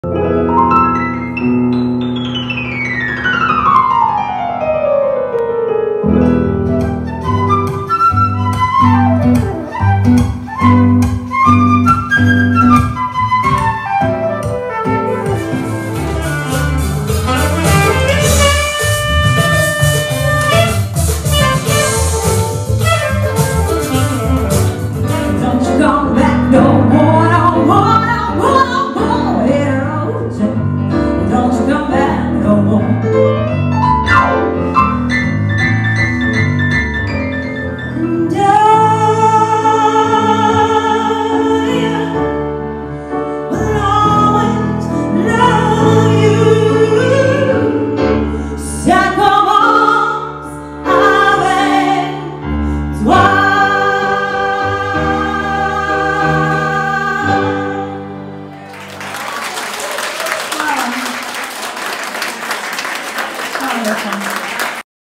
Lana Škrgatić live mini compilation
A few live moments from the concert in the beautiful Punat Gallery on the island Krk (Croatia)
Lana Škrgatić - piano, flute, sax, vocals